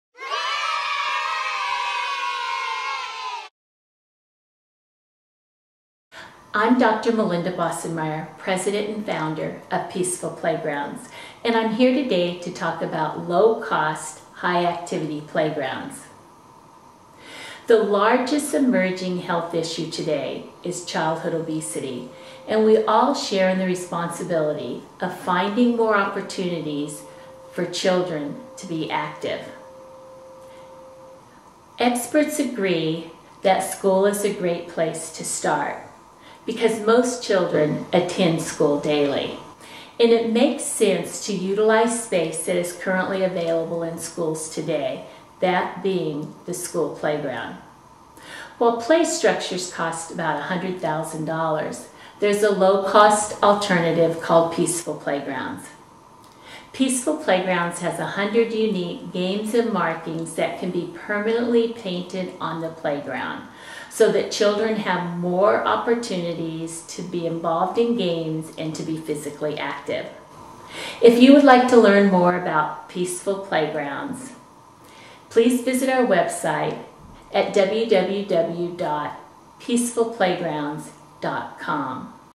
How to Create a Low Cost High Activity Playground
Low cost and high activity playground environments play an important role in getting children active as an intervention for childhood obesity.
for more information on Low Cost, High Activity Playgrounds.